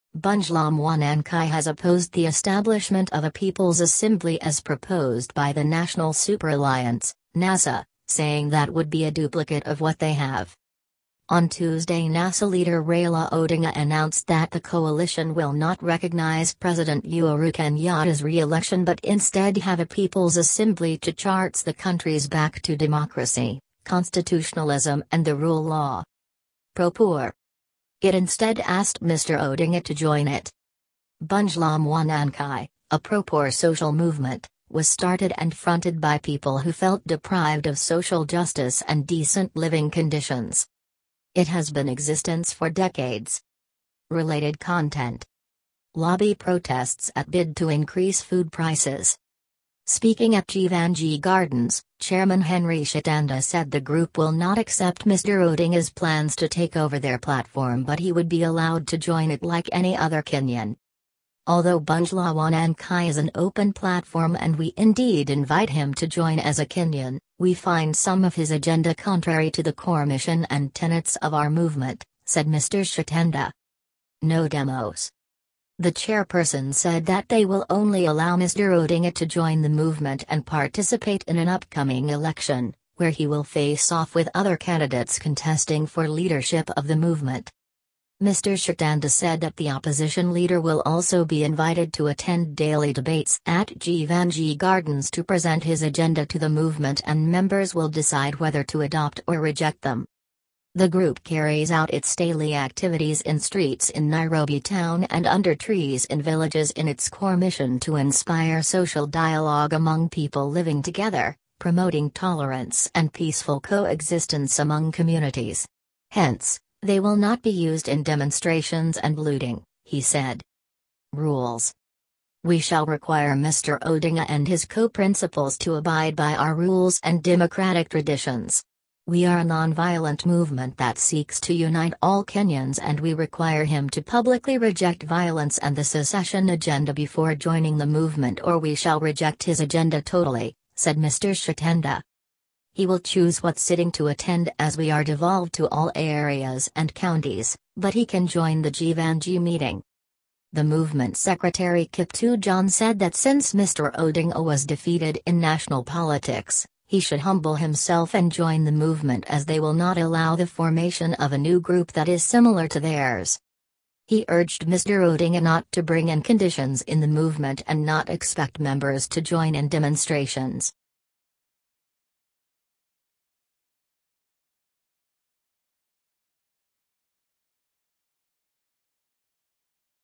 Bunge La Mwananchi opposes Nasa's People’s Assembly
Bunge La Mwananchi has opposed the establishment of a People’s Assembly as proposed by the National Super Alliance (Nasa), saying that would be a duplicate of what they have.

On Tuesday Nasa leader Raila Odinga announced that the coalition will not recognise President Uhuru Kenyatta’s re-election but instead have a People`s Assembly to charts the country’s back to democracy, constitutionalism and the rule law.

PRO-POOR

It instead asked Mr Odinga to join it.

Bunge la Mwananchi, a pro-poor social movement, was started and fronted by people who felt deprived of social justice and decent living conditions.

It has been existence for decades.

Related Content
Lobby protests at bid to increase food prices
Speaking at Jeevanjee Gardens, chairman Henry Shitanda said the group will not accept Mr Odinga’s plans to take over their platform but he would be allowed to join it like any other Kenyan.

“Although Bunge La Wananchi is an open platform and we indeed invite him to join as a Kenyan, we find some of his agenda contrary to the core mission and tenets of our movement,” said Mr Shitanda.

NO DEMOS

The chairperson said that they will only allow Mr Odinga to join the movement and participate in an upcoming election, where he will face off with other candidates contesting for leadership of the movement.

Mr Shitanda said that the opposition leader will also be invited to attend daily debates at Jeevanjee Gardens to present his agenda to the movement and members will decide whether to adopt or reject them.

The group carries out its daily activities in streets in Nairobi Town and under trees in villages in its core mission to inspire social dialogue among people living together, promoting tolerance and peaceful co-existence among communities. Hence, they will not be used in demonstrations and looting, he said.

RULES

“We shall require Mr Odinga and his co-principals to abide by our rules and democratic traditions. We are a non-violent movement that seeks to unite all Kenyans and we require him to publicly reject violence and the secession agenda before joining the movement or we shall reject his agenda totally,” said Mr Shitanda.

“He will choose what sitting to attend as we are devolved to all areas and counties, but he can join the Jeevanjee meeting.”

The movement secretary Kiptoo John said that since Mr Odinga was defeated in national politics, he should humble himself and join the movement as they will not allow the formation of a new group that is similar to theirs.

He urged Mr Odinga not to bring in conditions in the movement and not expect members to join in demonstrations.